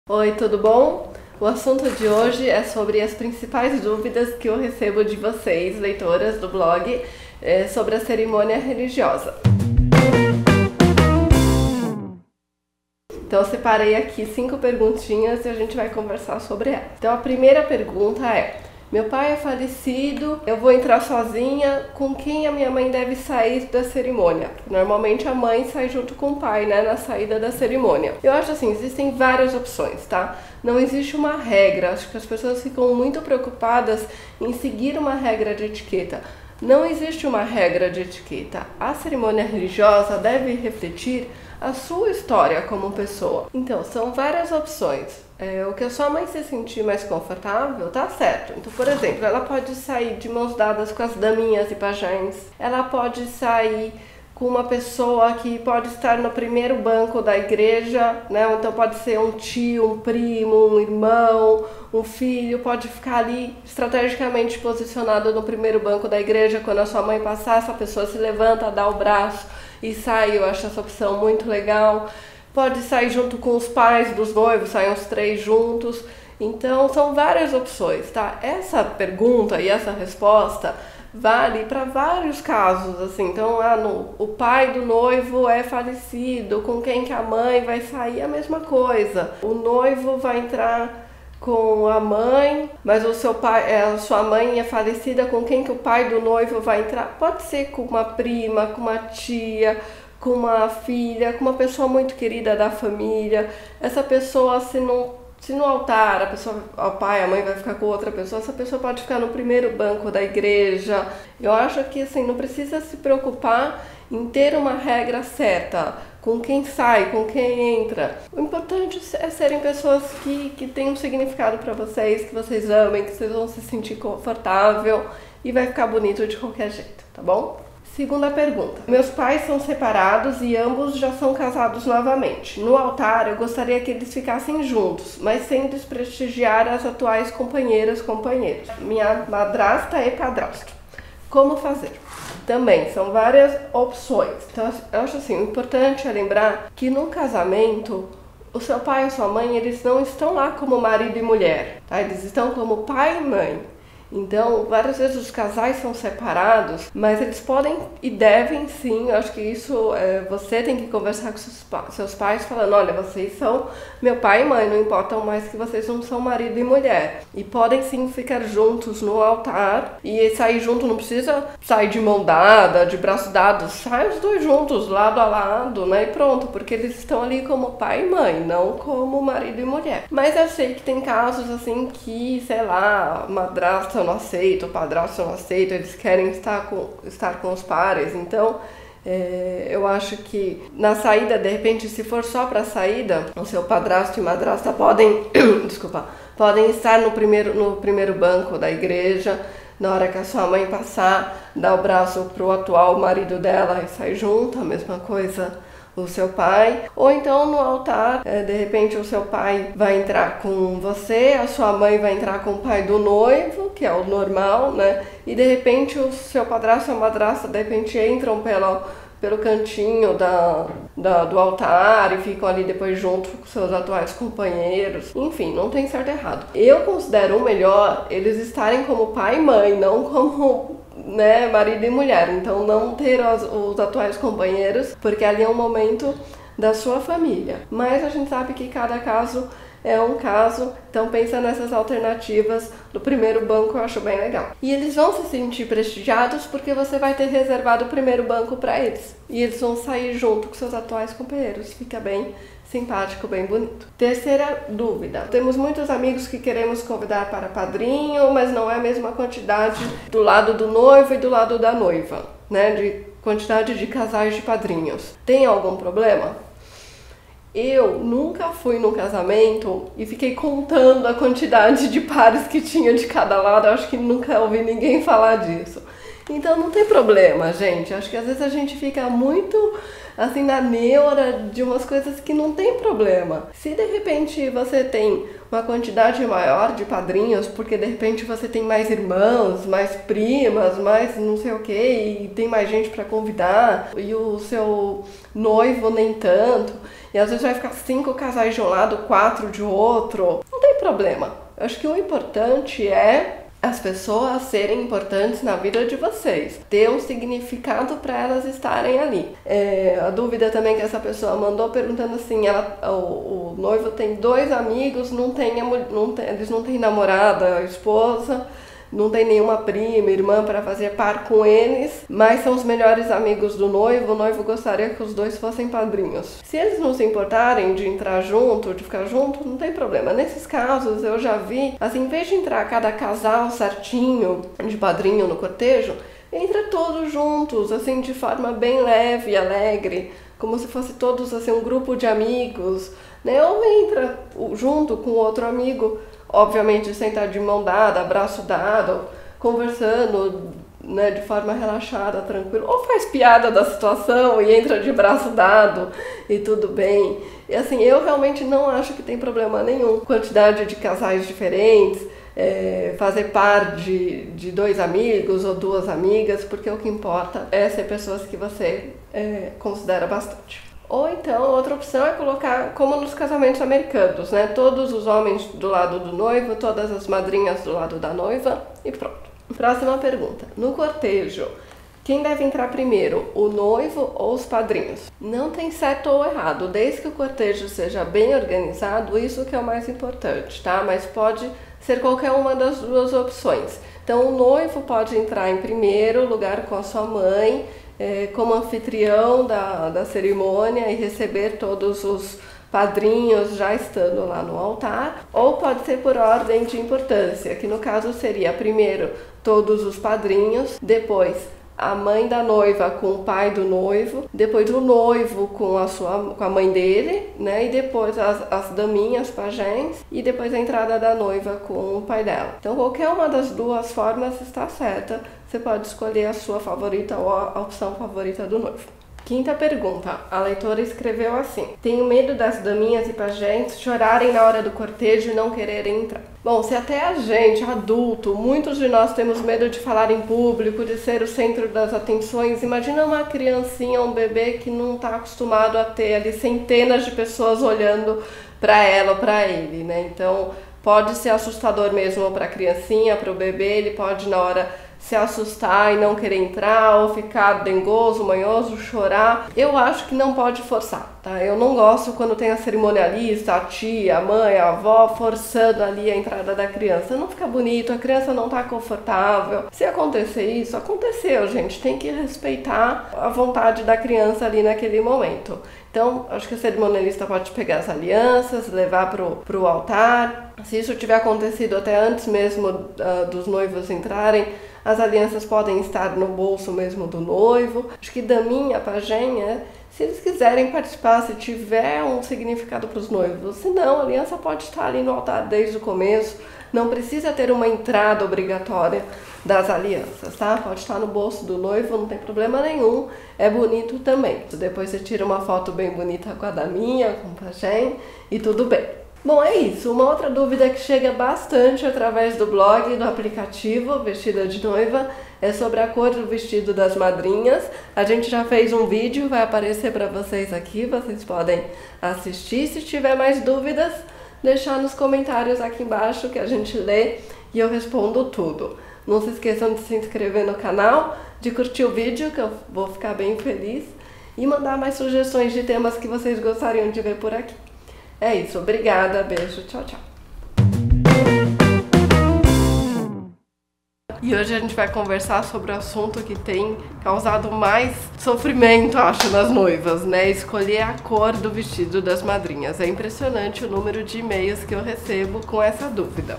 5 Dúvidas sobre Cerimônia Religiosa
Com quem a mãe saí quando o pai é falecido? No caso de pais separados, como fazer? Pode ter quantidade de casais diferente de um lado e de outro? Quem entra primeiro, o noivo ou padrinhos? E se as daminhas e pajens não querem entrar na hora? Todas estas dúvidas são respondidas neste vídeo!